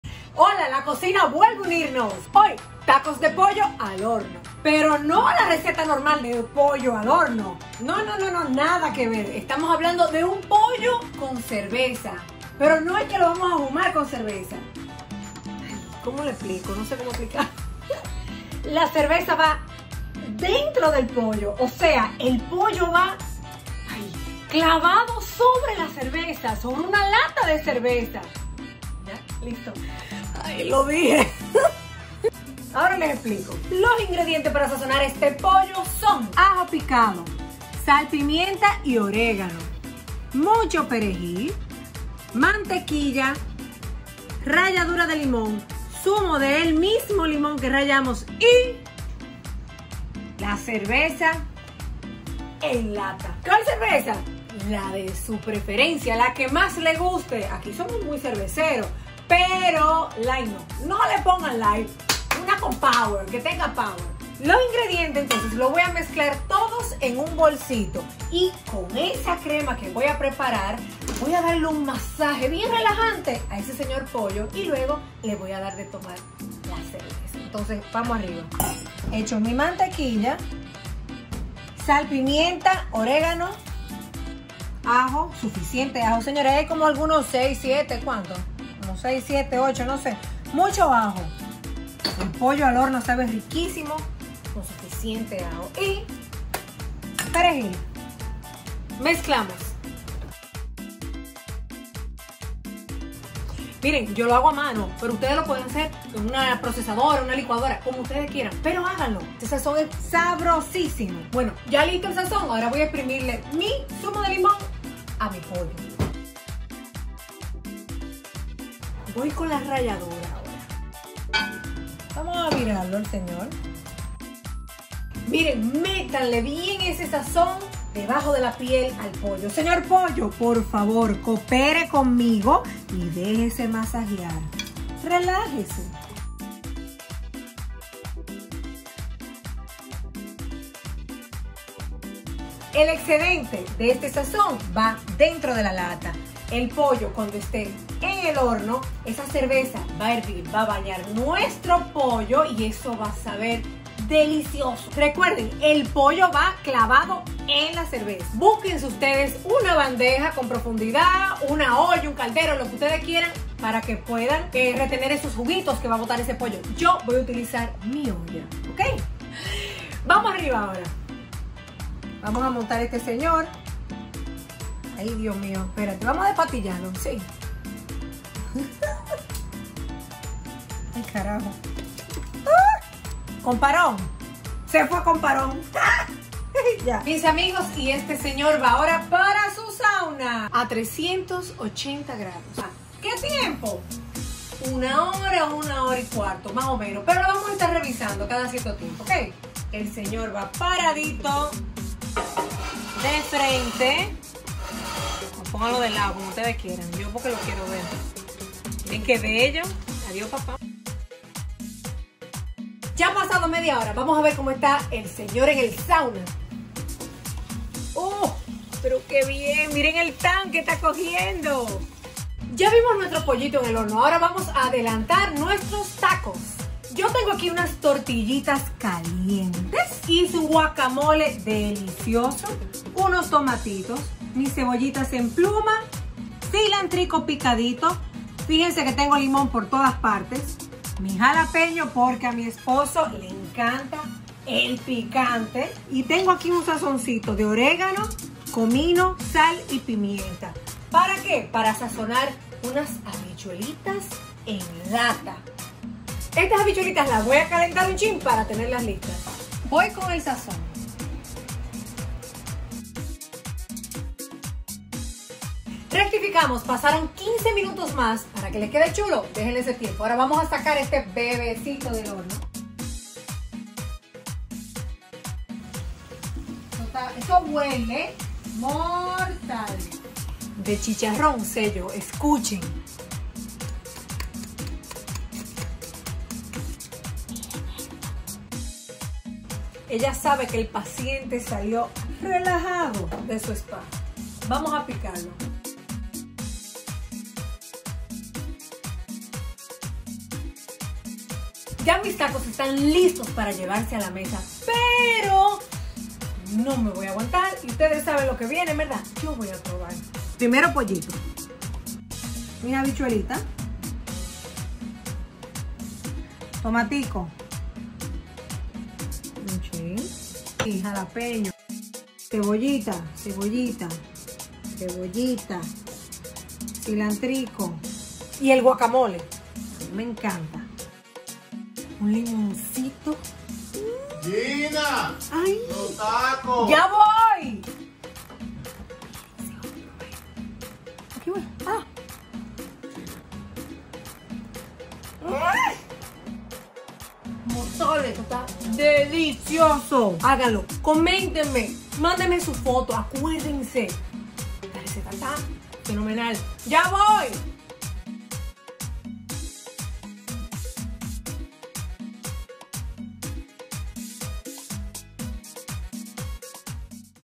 Tacos de pollo a la cerveza (pollo sentado, violado, clavado, sobre lata de cerveza)
Tacos de pollo a la cerveza

Esta sin duda se puede convertir en una de las recetas preferidas de ustedes, porque además de sencilla es muy rica!

Este pollo queda crocante por fuera y tierno por dentro, un espectáculo de verdad

Para esta preparación necesitan:

Un pollo entero

1 lata de cerveza( yo la abrí por arriba con la ayuda de un destapador)
Y de esa cerveza solo necesitan la mitad, así que la otra mitad se la pueden tomar

Ajo, algunos 8 dientes o mas (al pollo le va muy bien el ajo) también sirve pasta de ajo

Perejil en abundancia

Mantequilla una barra

Limón , la ralladura de su cascara y el zumo del mismo

Sal, pimienta y oregano

Para los tacos necesitan

Tortillas de maíz

Un buen Guacamole

Cebollita morada cortada como a ustedes les guste

Tomates picados en cuadritos

Cilantrico

Jalapeño

Habichuelas rojas

Oregano+comino+sal+pimienta

Limones cortaditos